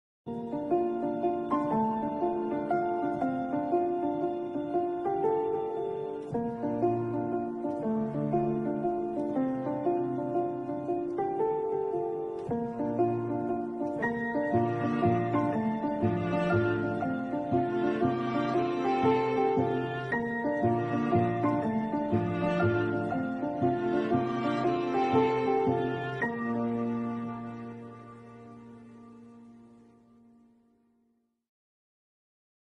The funny puppy shepherd dog accompanying the herd meets the sheep on the first mission day
We've selected the best of the funniest and cutest animals videos that will make you laugh all day long.
cats and dogs and other animals